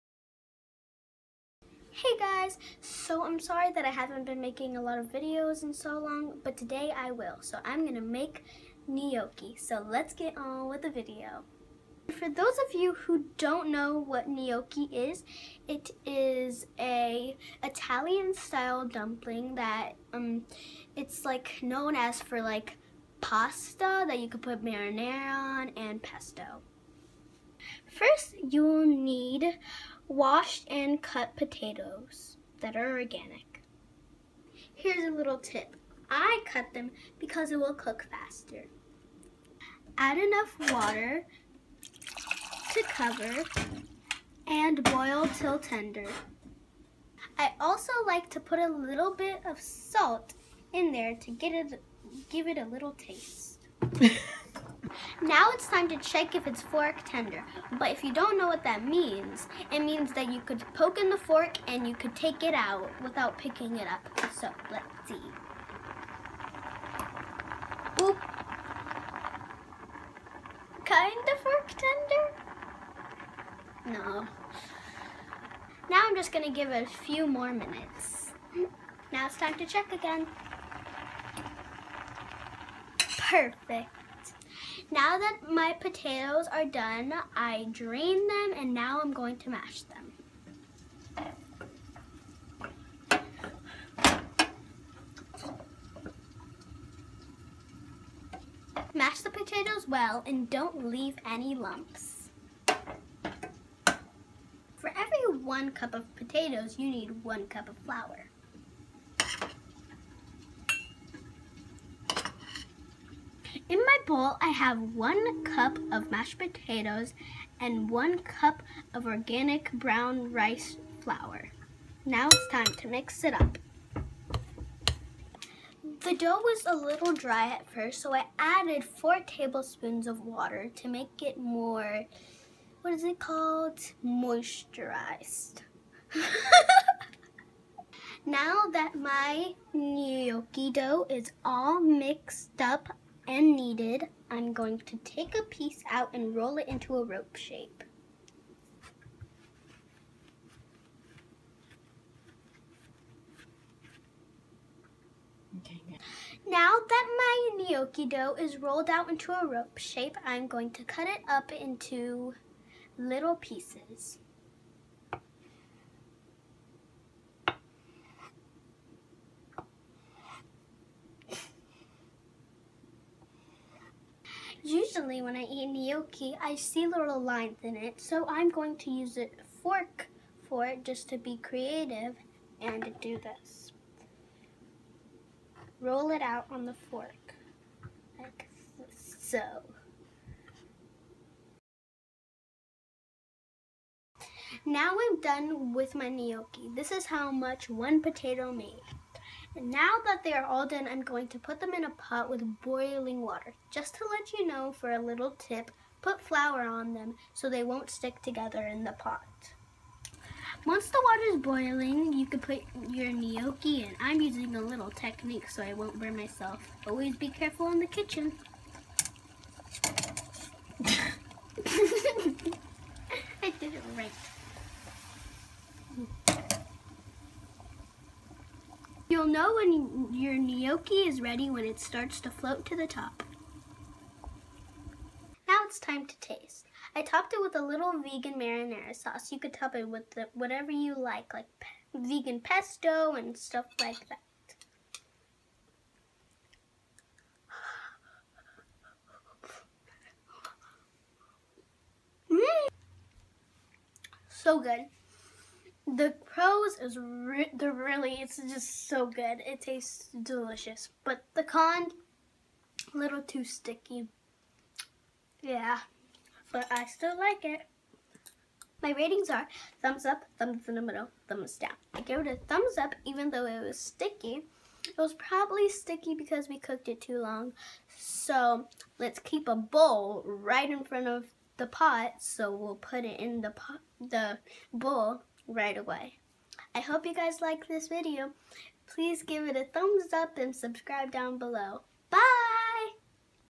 Vegan Potato Gnocchi
All you will need is:
1 cup potato (I used organic russet)
1 cup flour (I used organic brown rice flour)
1 teaspoon salt